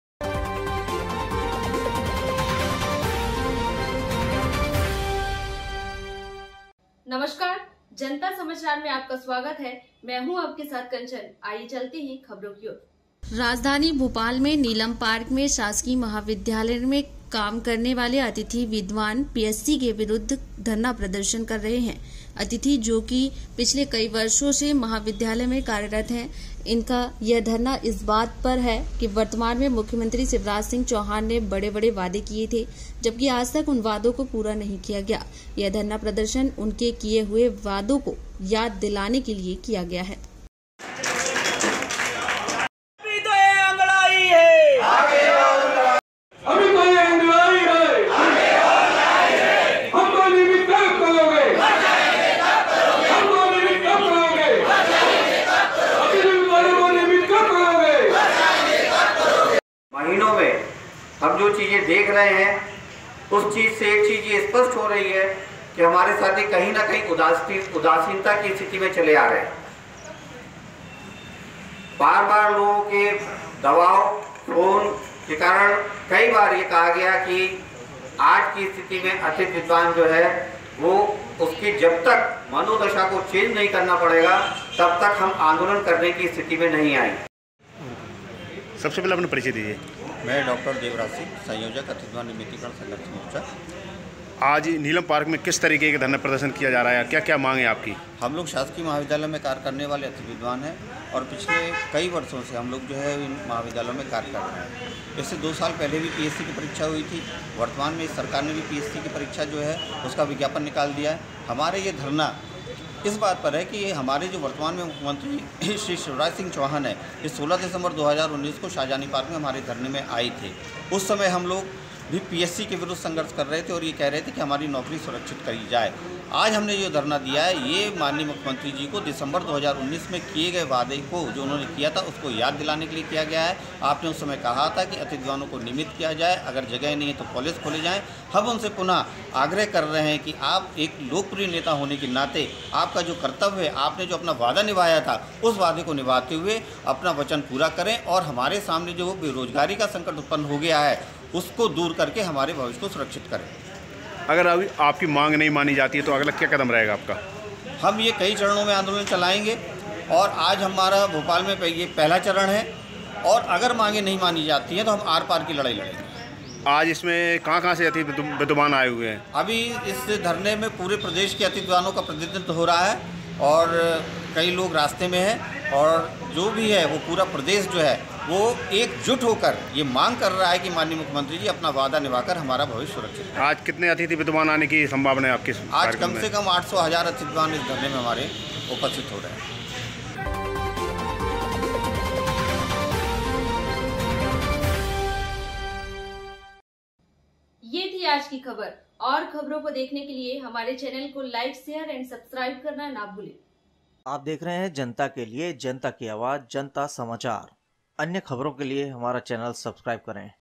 पीएससी के विरुद्ध धरना प्रदर्शन30/01/2023#MP#JantaSamachar.news#hindi_news_Breaking
राजधानी भोपाल मे नीलम पार्क मे शासकीय महाविद्यालय में काम करने वाले अतिथि पीएससी के विरुद्ध  किया धरना प्रदर्शन अतिथि  जो की पिछले कई वर्षों से महाविद्यालय में कार्यरत है इनका यह धरना इस बात पर है वर्तमान में मुख्यमंत्री शिवराज सिंह चौहान ने बड़े-बड़े वादे किए थे  जबकि आज तक उन वादों को पूरा नहीं किया गया यह धरना प्रदर्शन उनके किए हुए वादों को याद दिलाने के लिए किया गया है

पीएससी के विरुद्ध धरना प्रदर्शन